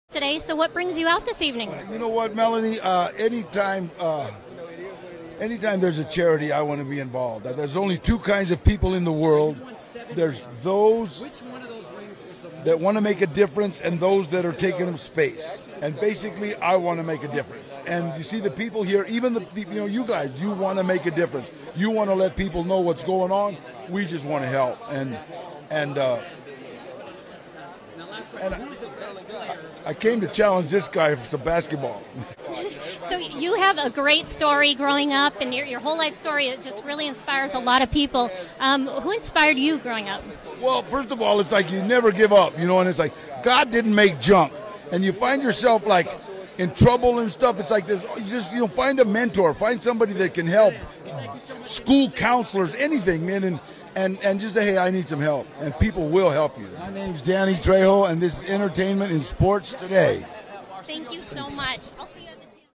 Danny Trejo's Advice on Being Inspired
"God doesn't make junk", Danny Trejo tells us during an interview on the red carpet for Sports Spectacular.  Danny "Machete" Trejo explains who and what inspired him and offers some words of wisdom.  It's difficult to imagine that this kind, humble, generous man is the same person who was hooked on drugs by the age of 12, in jail by 15 and eventually at only 23 years old, sentenced to San Quinton Prison.  As word spreads of his highly anticipated documentary "Inmate #1: The Rise of Danny Trejo" (2018 projected released), we all look forward to hearing the whole story about how Trejo "rose" above it all to become a producer, an award-winning actor, a mentor and owner of several food and restaurant businesses (Including the incredibly addictive Trejo's Tacos). Danny Trejo is a true star who can teach and inspire us all!